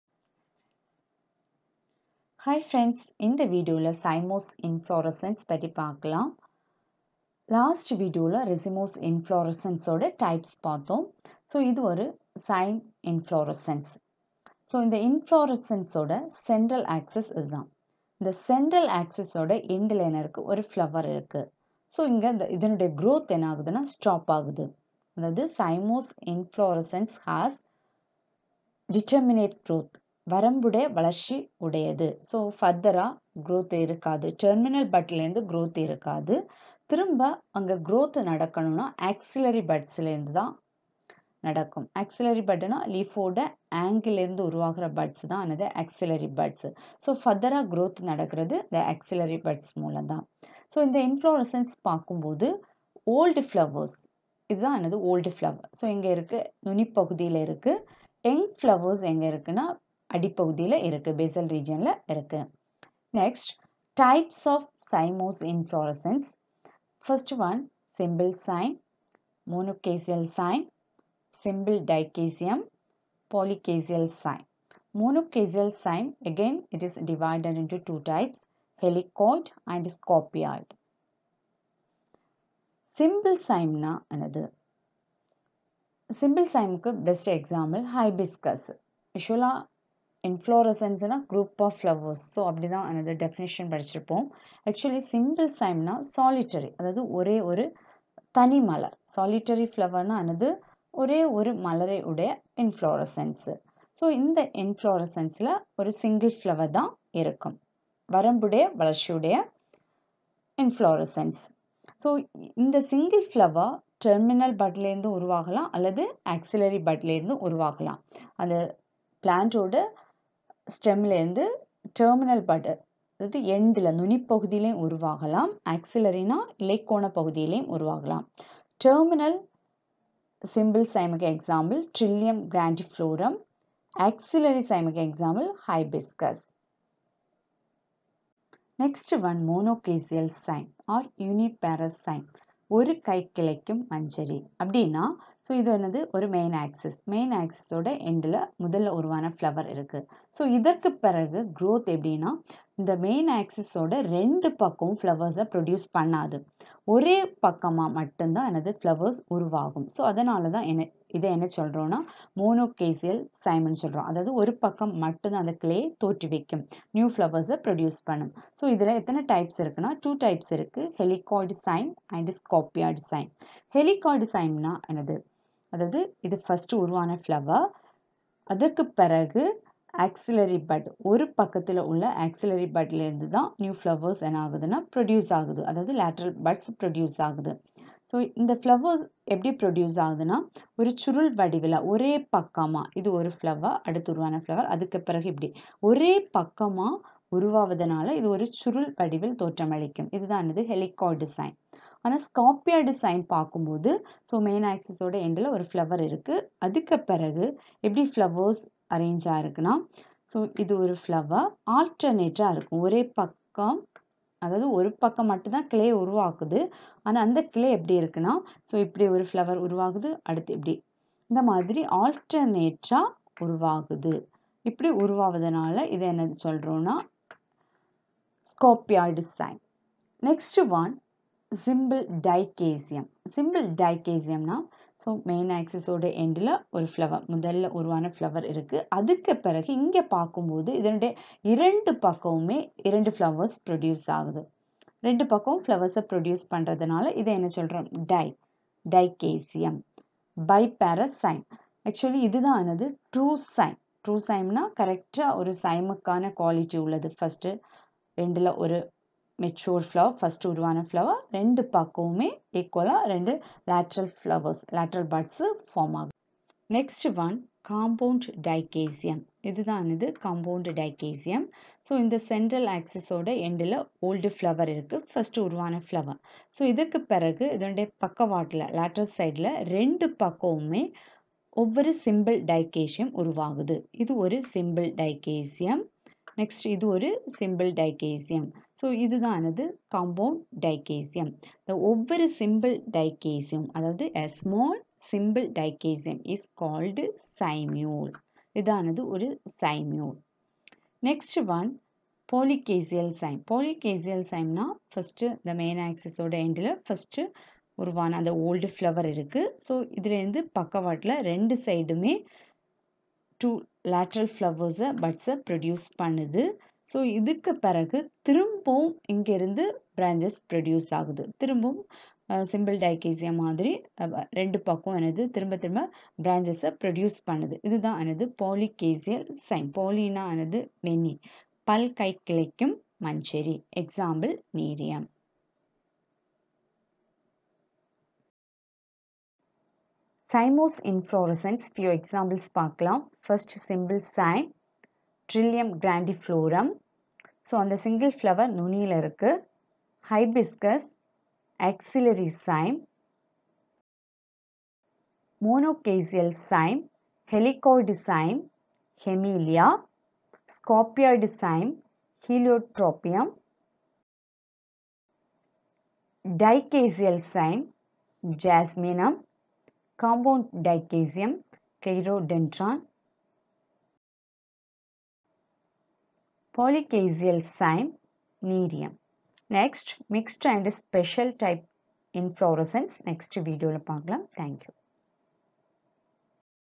Types of Cymose inflorescence /Reproductive morphology /in tamil
Types of cymose inflorescence /simple cyme/dichasial cyme/monochasial cyme/compound dichasium/polychasial cyme